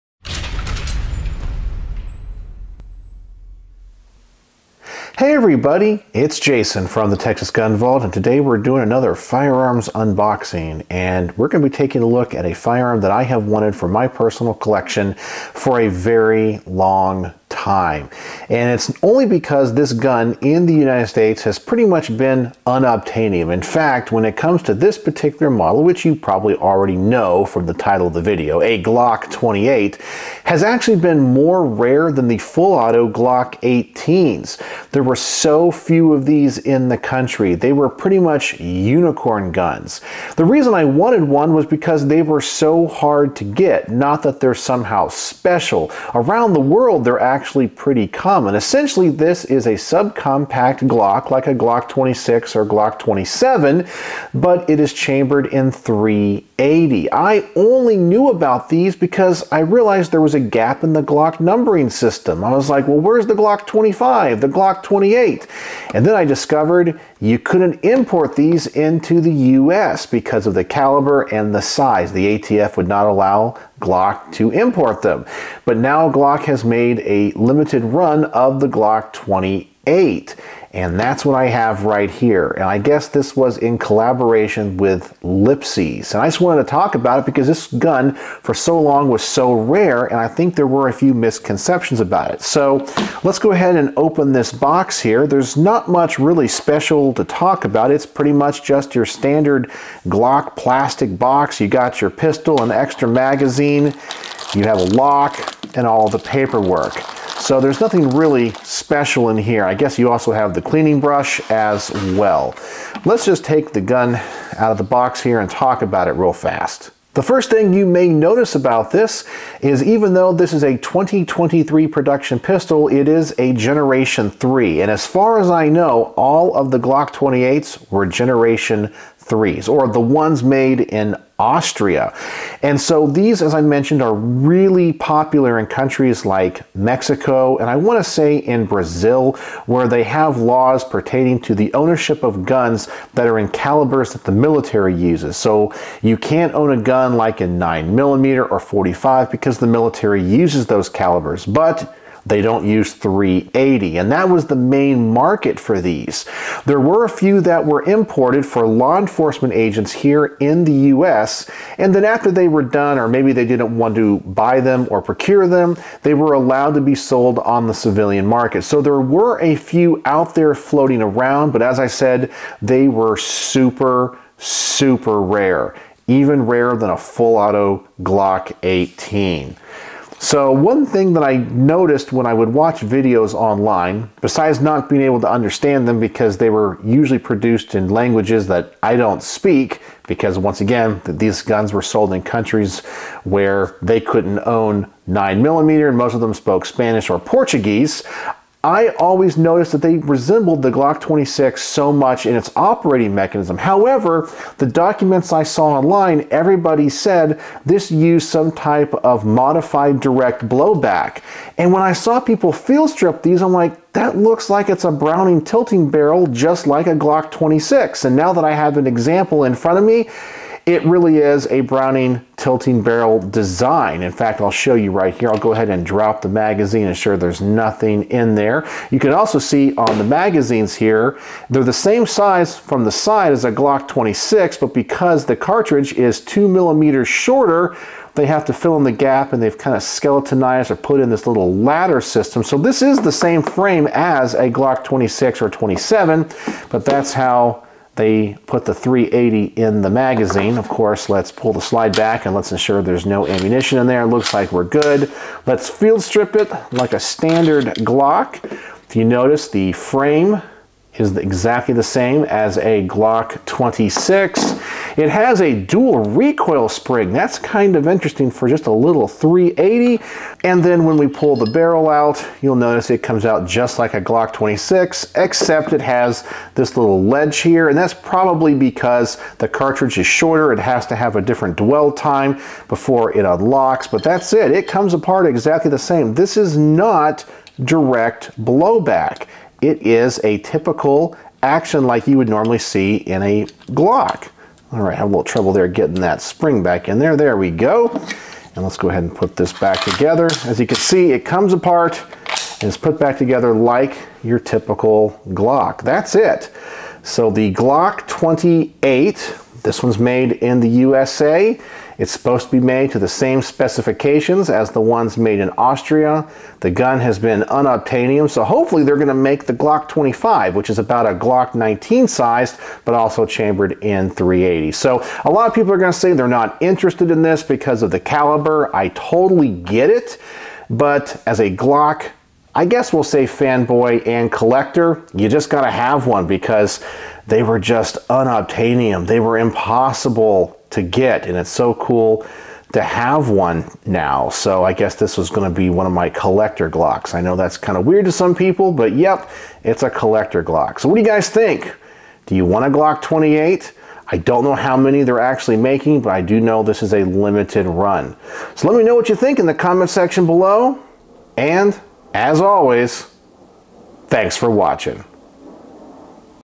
Unboxing: Glock G28 - The .380 Glock that could never be imported
In this firearms unboxing video I unbox a Glock pistol I have wanted ever since I found out it existed. Not because of how it worked but because there were so few of them in the U.S. Now I have one as Glock has made a limited run of U.S. versions. I am very excited to have this in my collection.

Mailing Address -